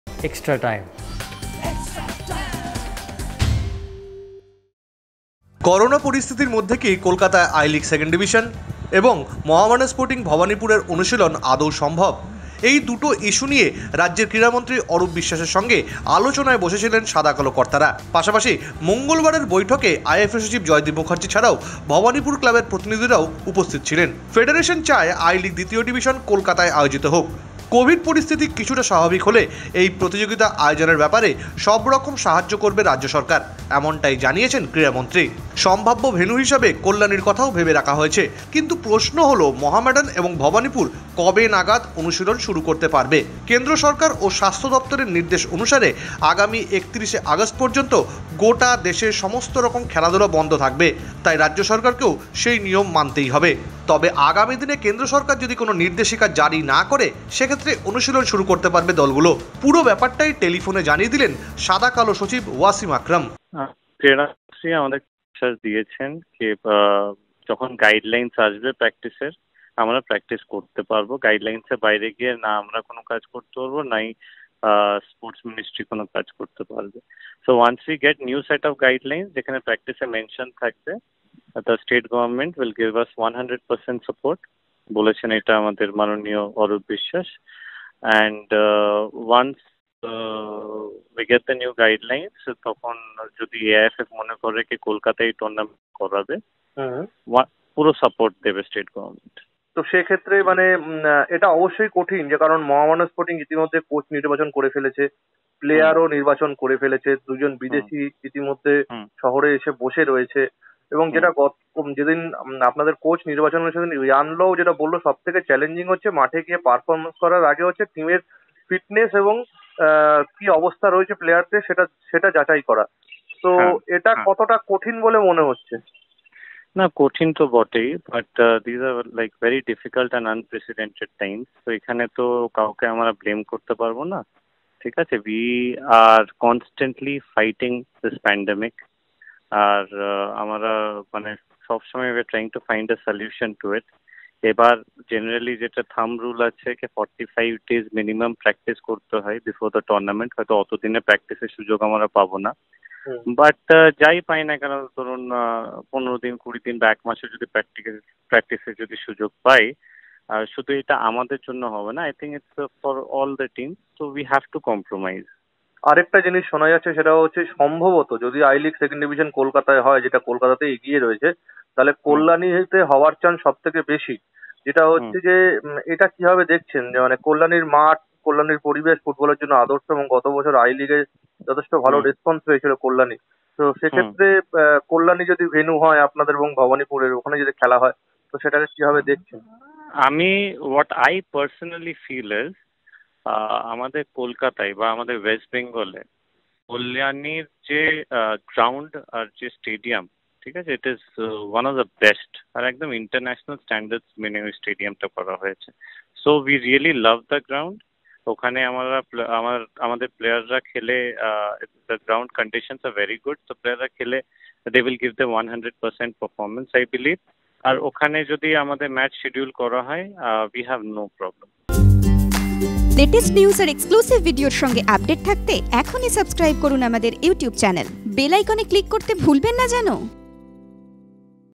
Mohammedan Sporting অনুশীলনে নামতে পারবে কবে? অরূপ বিশ্বাস কী বলছেন?
Story Producer & Voice: Sabyasachi Bagchi